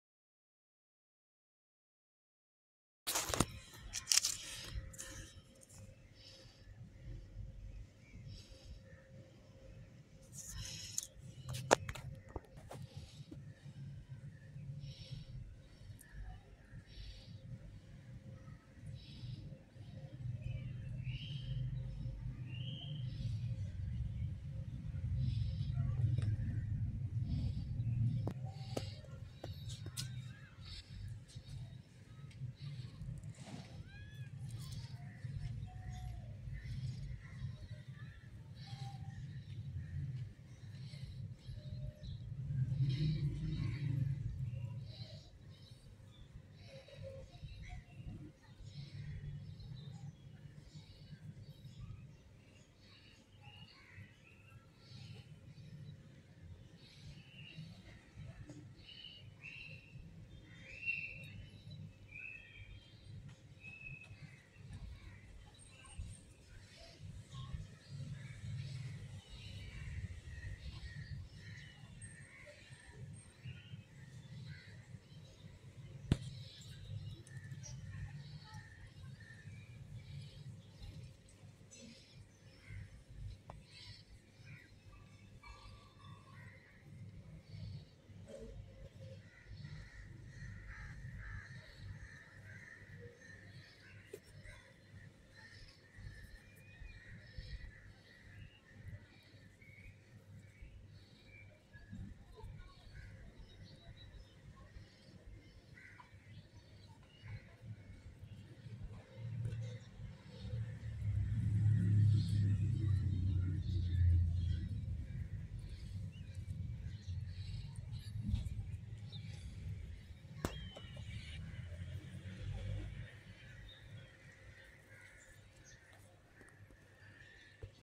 Easy to Guide Growing Sunflower @BirdsNBuds
🌻 **Ultimate Guide to Growing Sunflowers!** 🌻 Ready to brighten up your garden with the vibrant beauty of sunflowers? In this easy-to-follow guide, we’ll walk you through every step, from selecting the right seeds to planting and nurturing these stunning giants! Perfect for beginners and seasoned gardeners alike, discover tips on sun exposure, soil quality, watering schedules, and pest control to ensure a bountiful bloom. 🌞🌱 Join our blooming community and unleash your inner gardener! Let's grow together! 💚